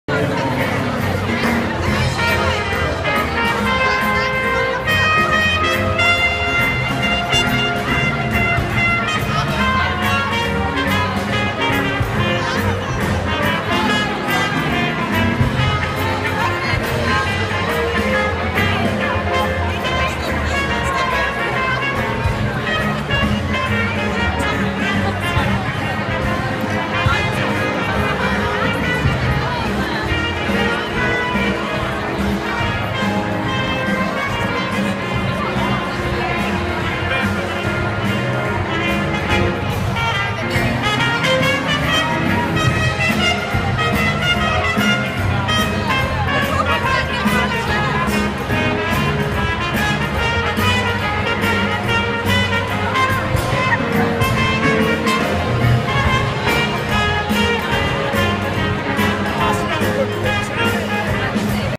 Bitzofgigz Gin Festival music
Band playing at Leeds Town Hall whilst sampling some fine gins not least the sloe gin and prosecco concoctions